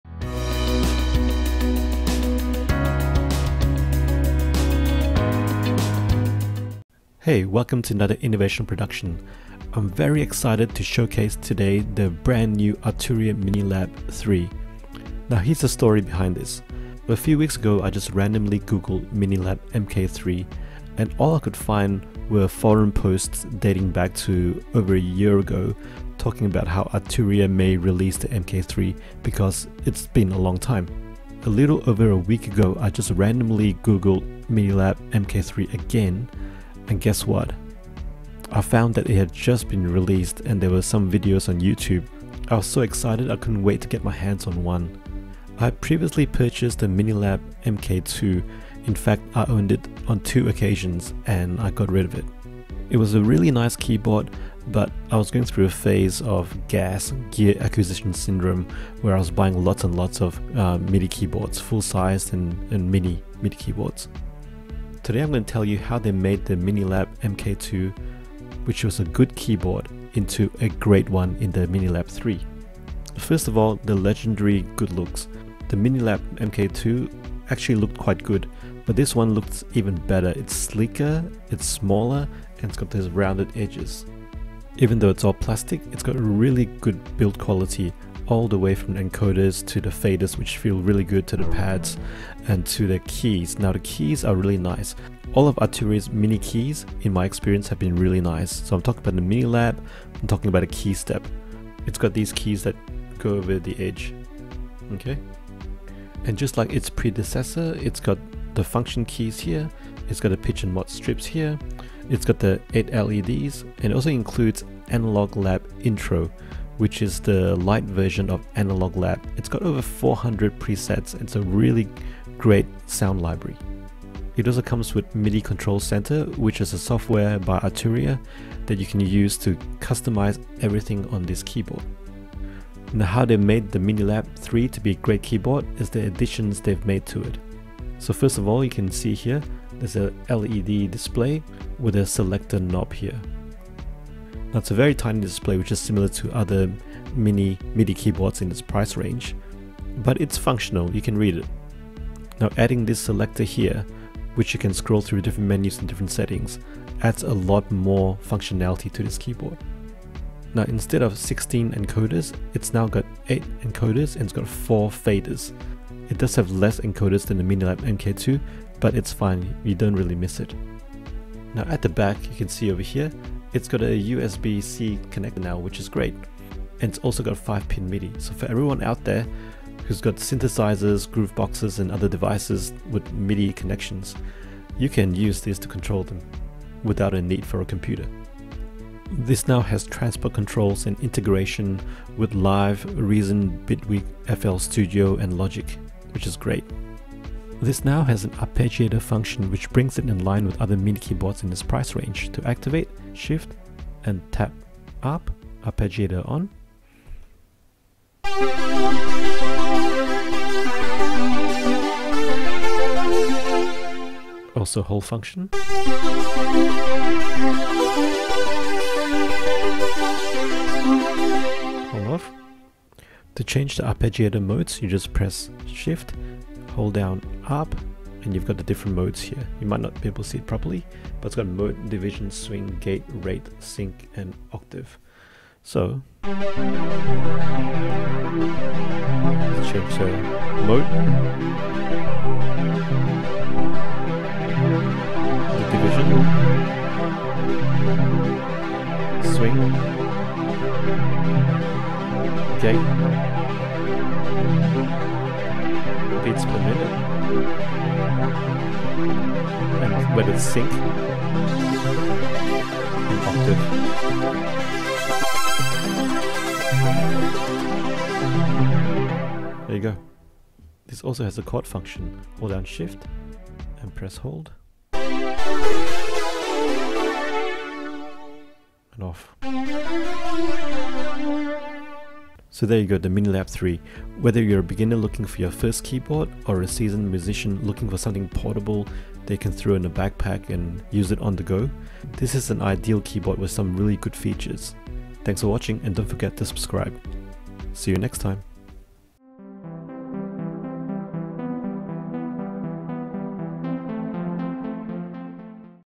Arturia Minilab 3 (MK3) - A Good Keyboard Becomes a Great Keyboard iNNovational Showcase Review
This is the upgrade we have been waiting a long time for. The Minilab MK2 was a nice keyboard, but the Minilab 3 with its multiple additions makes this one of the most compelling mini MIDI keyboard buys at the moment.

Amazon USA
Amazon AU

CONTENTS
0:00 INTRO
1:13 "GOOD LOOKS"
1:25 BUILD QUALITY
1:33 KEYBED
1:46 WHAT'S THE SAME
2:12 WHAT'S NEW?
2:18 OLED SCREEN
2:40 LESS ENCODERS, ADD FADERS
2:51 USB-C
2:56 MIDI DIN CONNECTION
3:08 DAW INTEGRATION
3:17 ARPEGGIATOR + LATCH
3:43 ARP MODES
4:30 CHORD FUNCTION
4:42 CONCLUSION